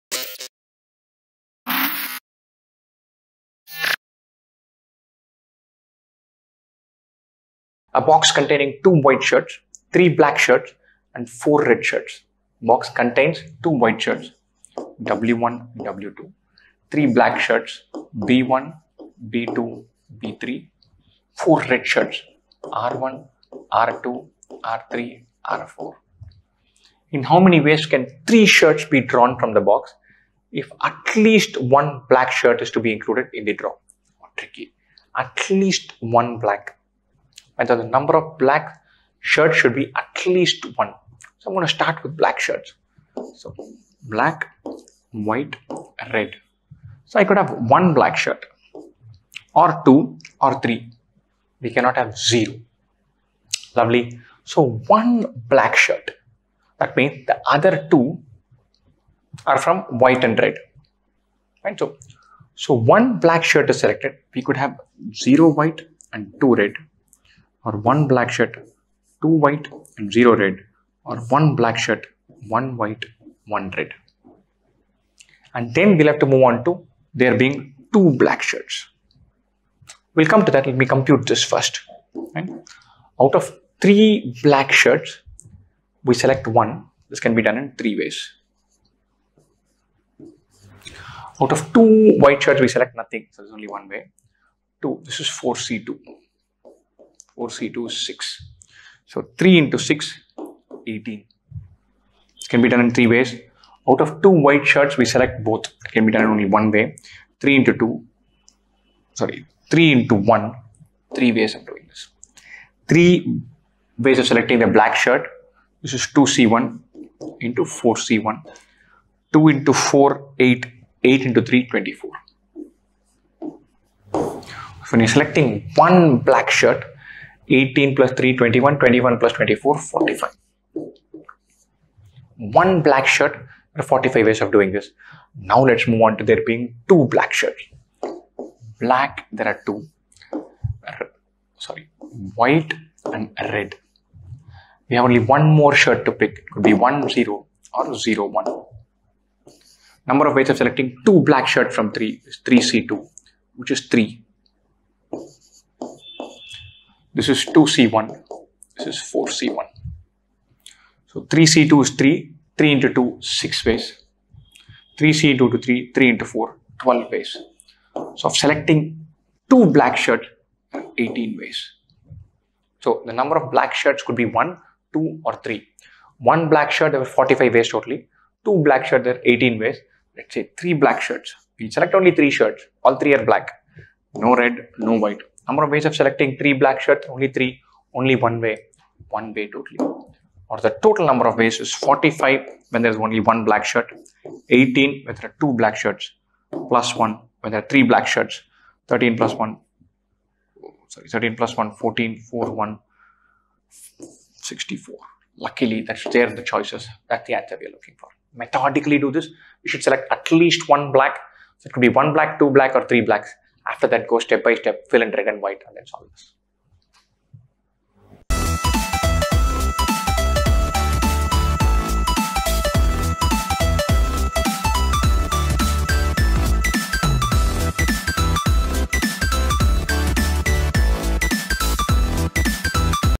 Box of shirts | IPMAT Rohtak Previous Year Papers | IPMAT 2020 Solutions | 2IIM IPMAT Preparation
A box contains 2 white shirts, 3 black shirts, and 4 red shirts. In how many ways can 3 shirts be drawn from the box, if at least one black shirt is to be included in the draw?

A. 60
B. 40
C. 64
D. 82

Best wishes for IPMAT!

2IIM's Online Course for IPMAT 2021 - ipm.2iim.com